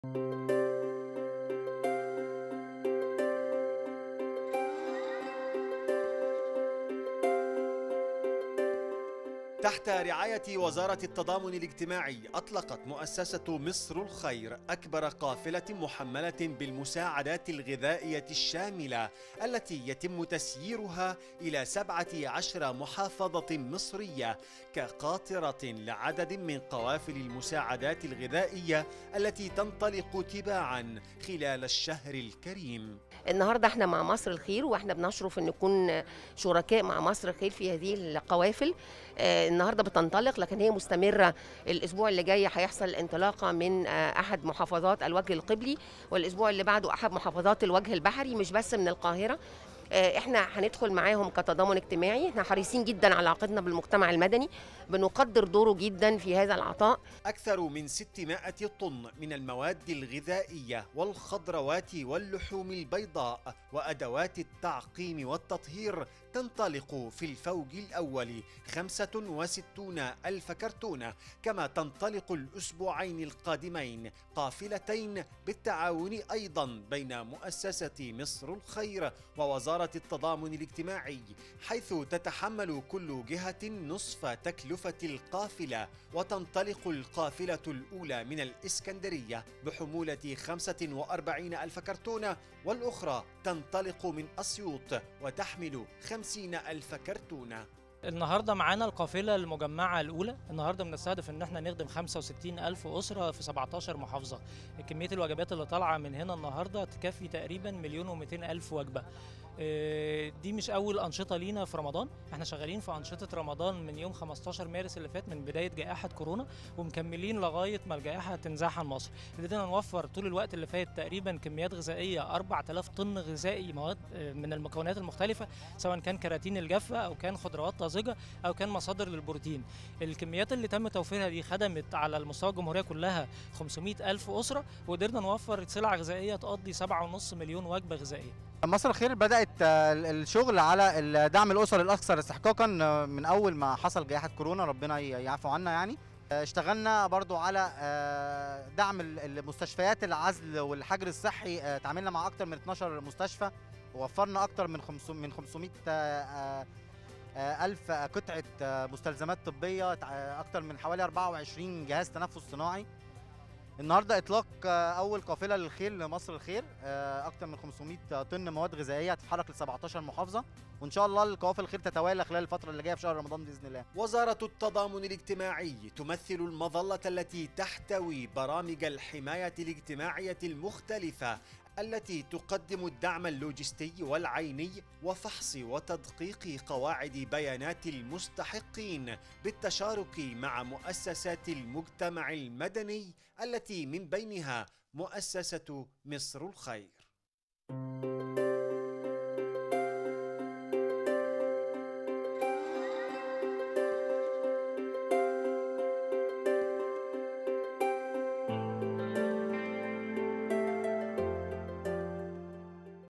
الآن | مصر الخير تطلق أكبر قافلة مساعدات غذائية للأسر المستحقة بأكثر من 600 طن من المواد الغذائية
3 مايو 2020